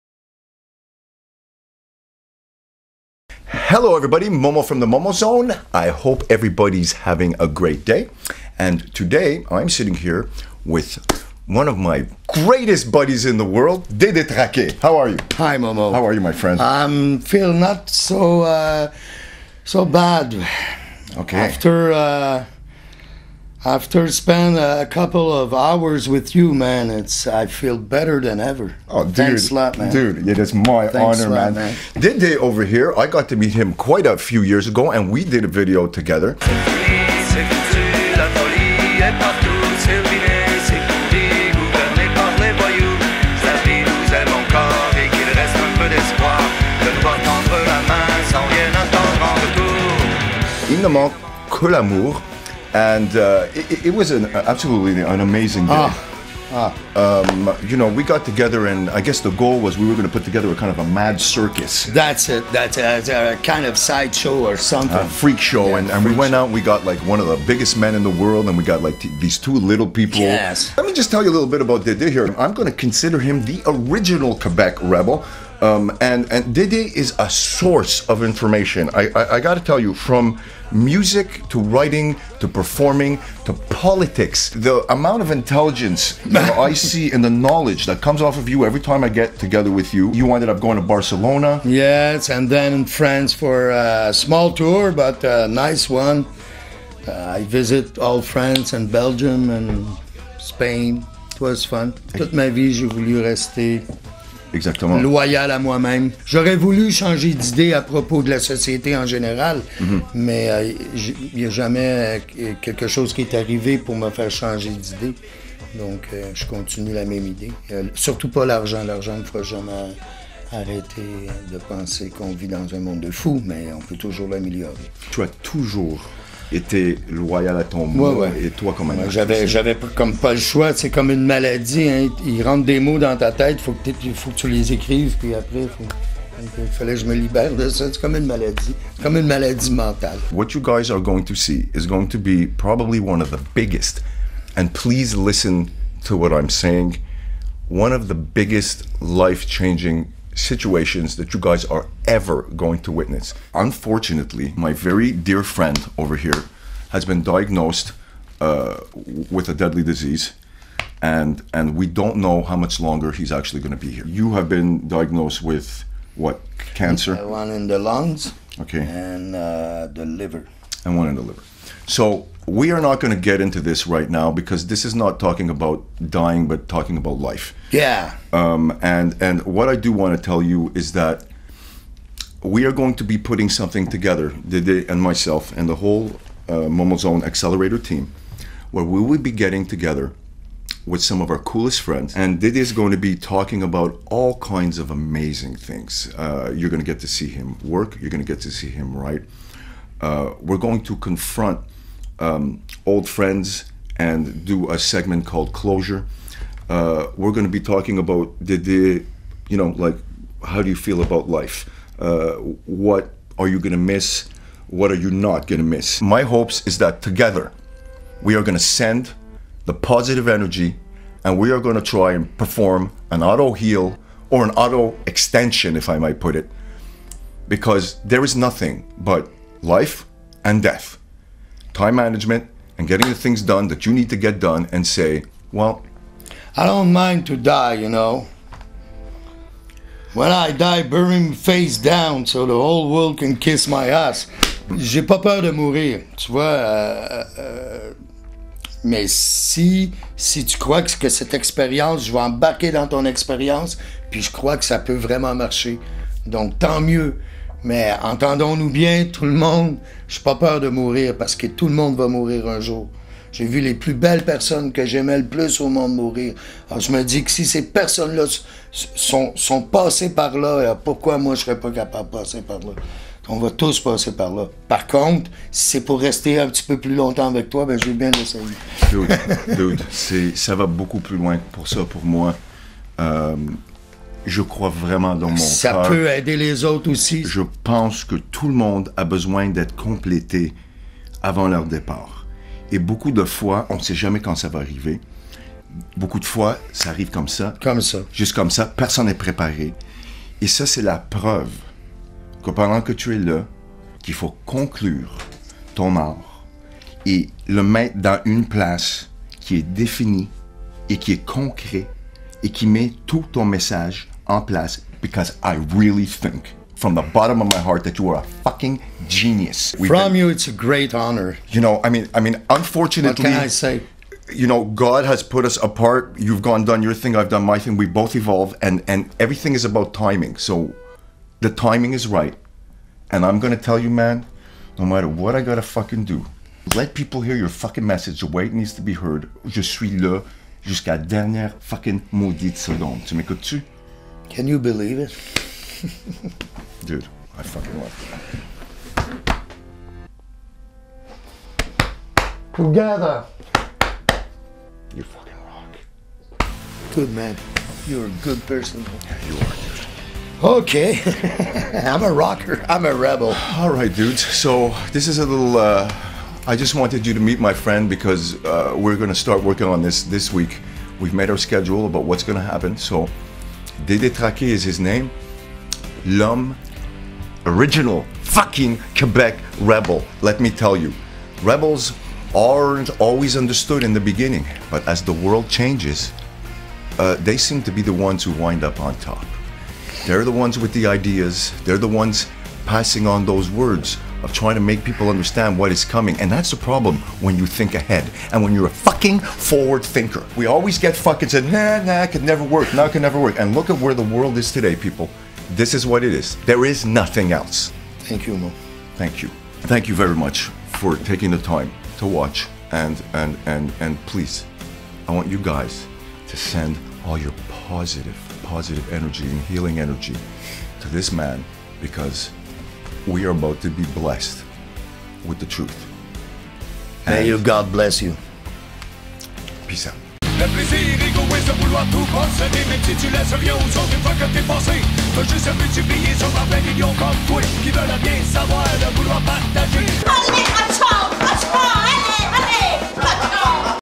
Rebel till the end
I got together with my friend dede trake for xelerator 2  to talk about music,toys,politics life and death.Dede has been diagnosed  with  2 kinds of cancer  and he might not be with us for much longer.Follow the upcoming episodes that you wont soon forget.Please send your positive healing vibes for him to cure or extend his stay.
momo